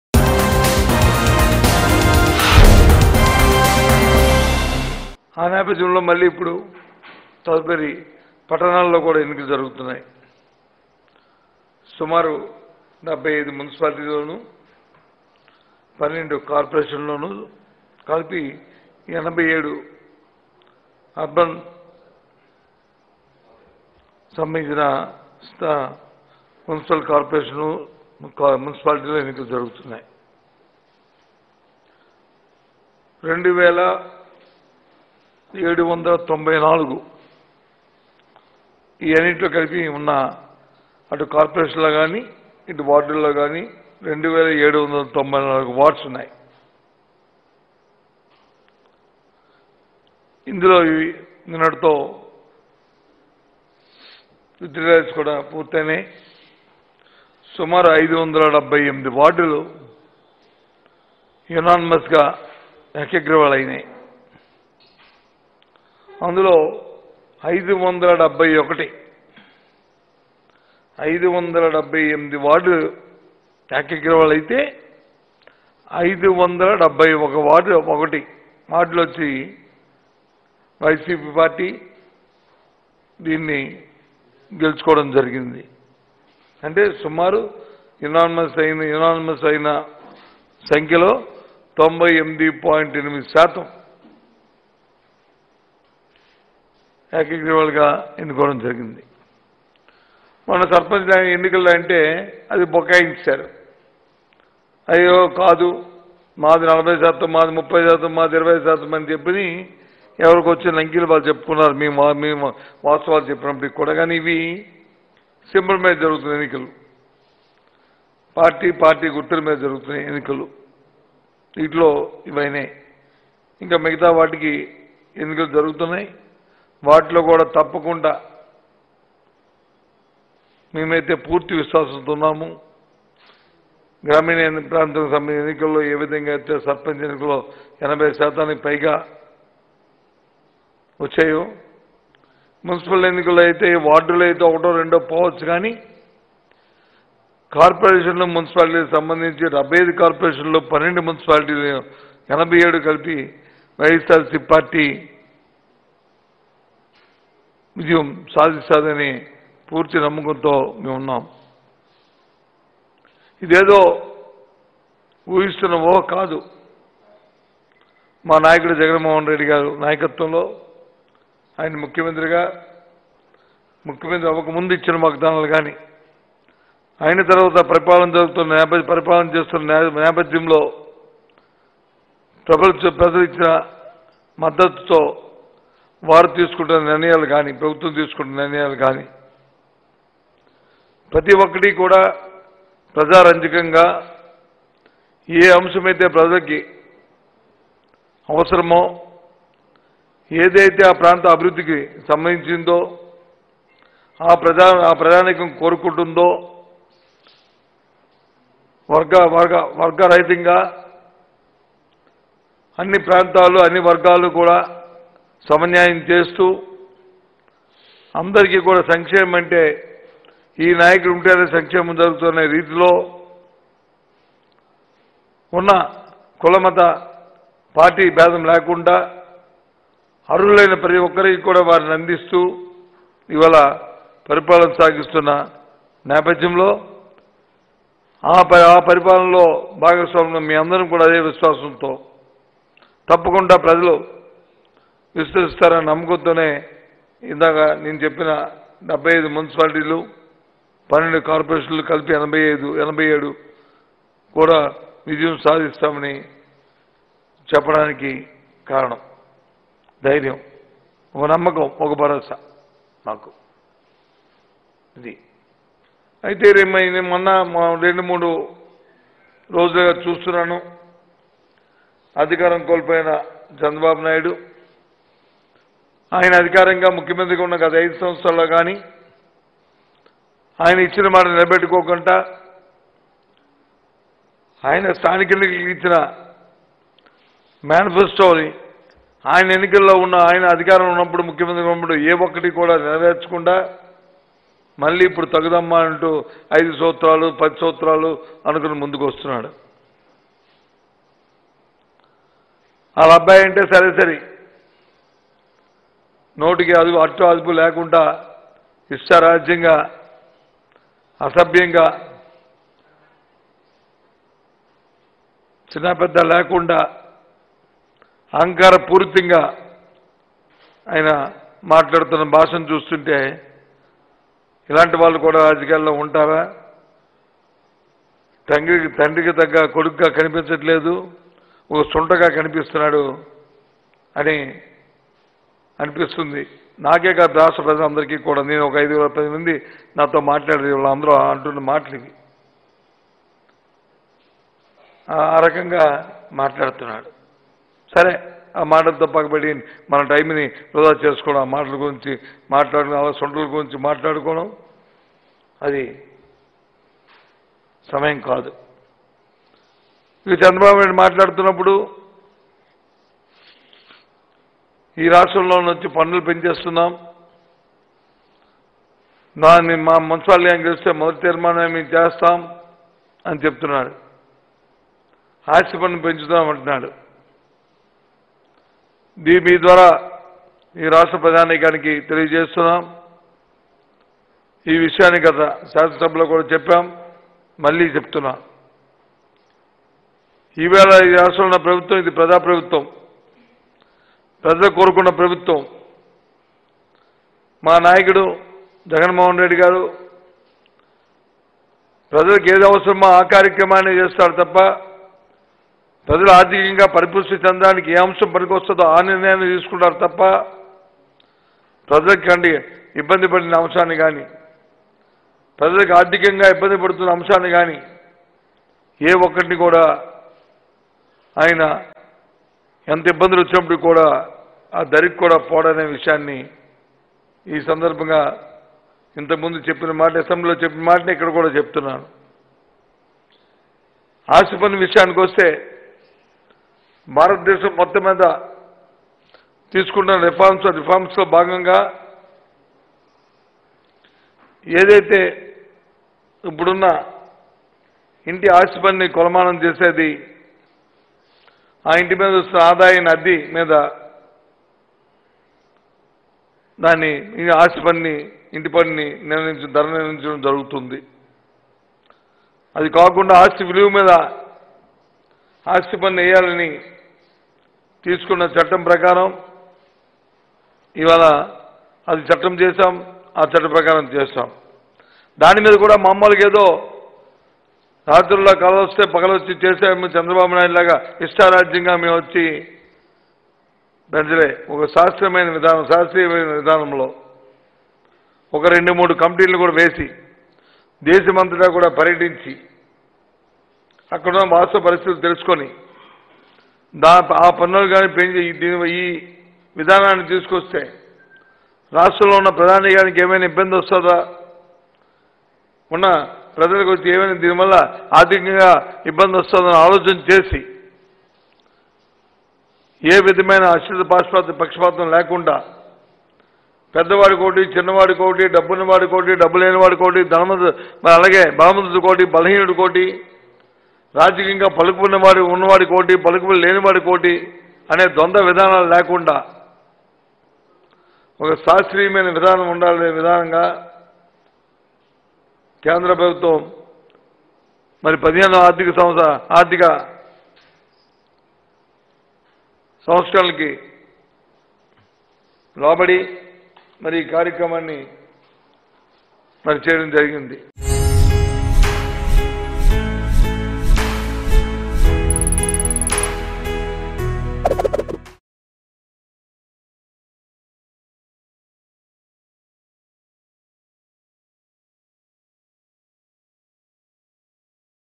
Botsa Satyanarayana Expresses Confidence Over YCP Victory In AP Municipal Elections | Mango News
Botsa Satyanarayana Expresses Confidence Over YCP Victory In AP Municipal Elections.